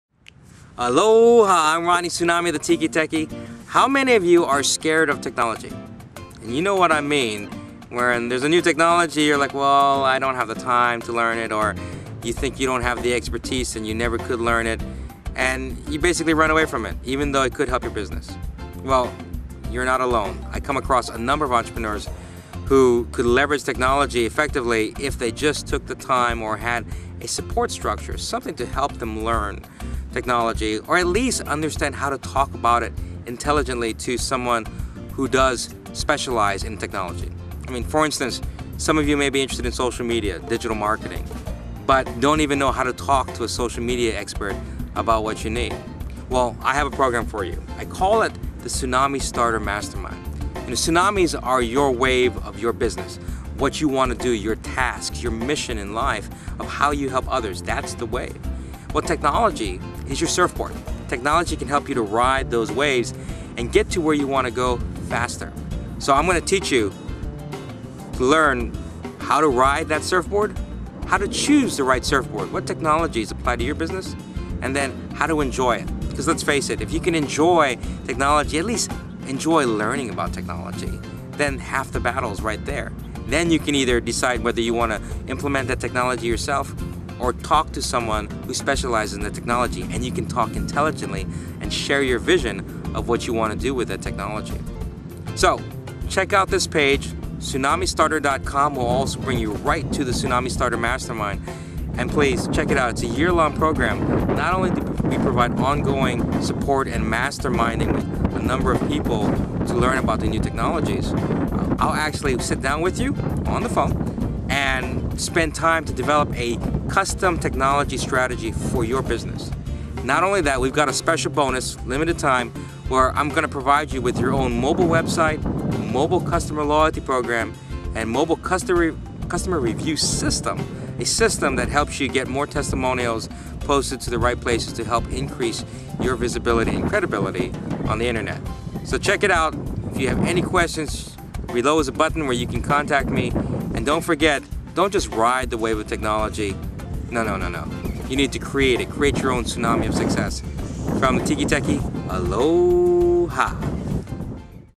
The Tsunami Starter Mastermind
Ronnie Tsunami introduces his Tsunami Starter Mastermind, a 12-month program designed to help business owners become tech-savvy entrepreneurs who embrace technology and understand how to effectively leverage it to save time, save money and make money.